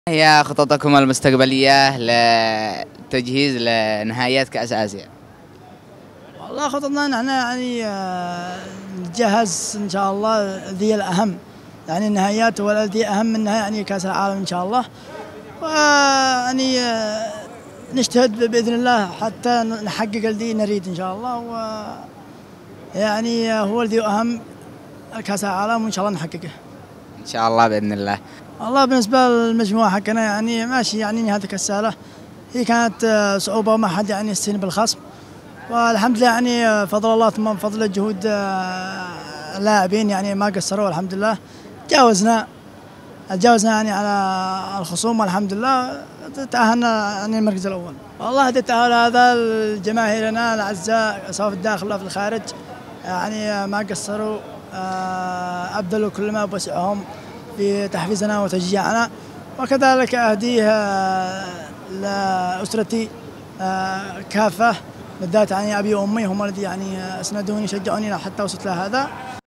كابتن منتخب الناشئين عصام ردمان يحاور في لقاء رياضي النجم انور الطريقي
كابتن منتخب الناشئين عصام ردمان في لقاء رياضي مع النجم انور الطريقي

تليجرام